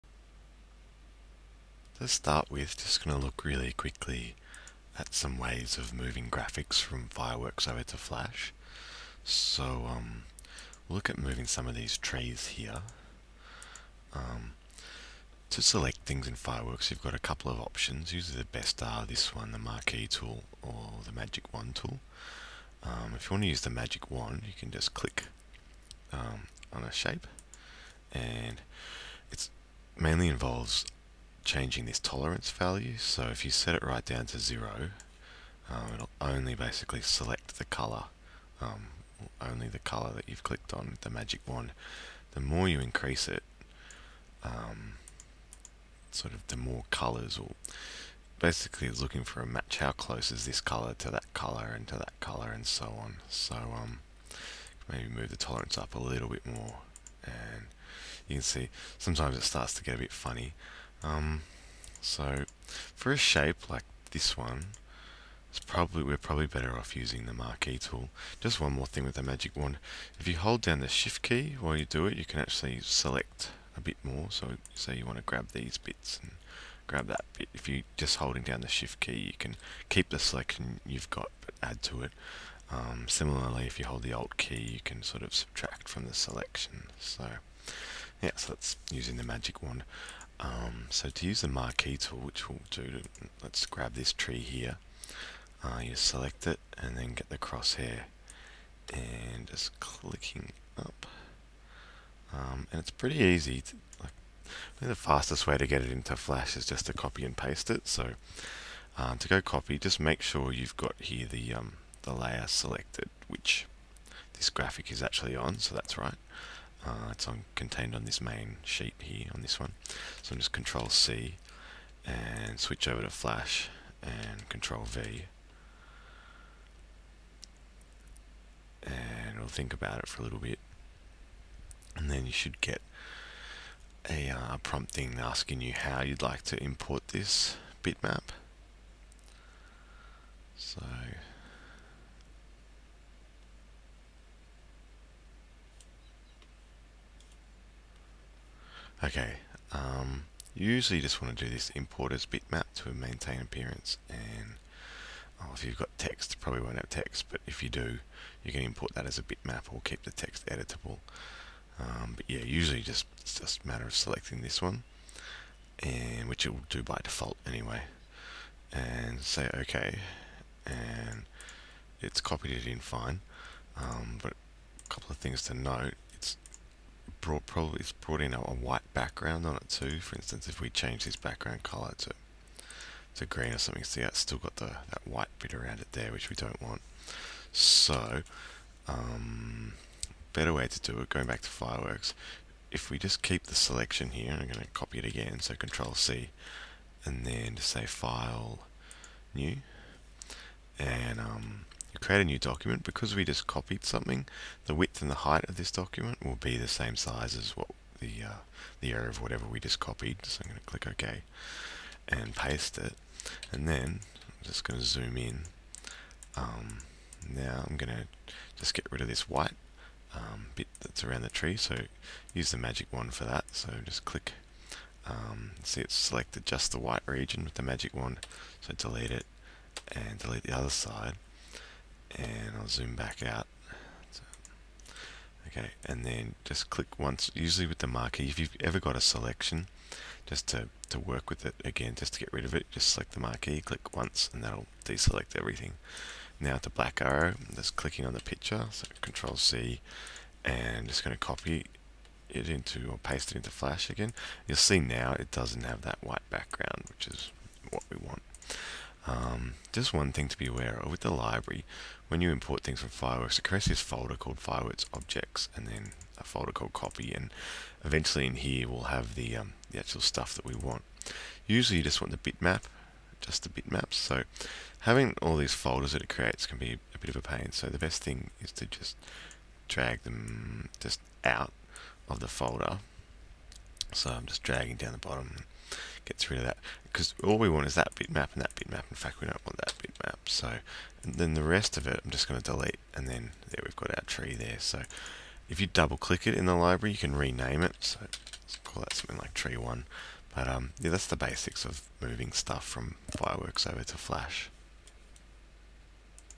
Flash RPG tutorial (Part 1)
This is a tutorial to create an RPG game using Macromedia flash CS3. this part covers how to remove sprites from fireworks to flash. This tutorial was created by our computing studdies extended teacher, Mr. Falkland.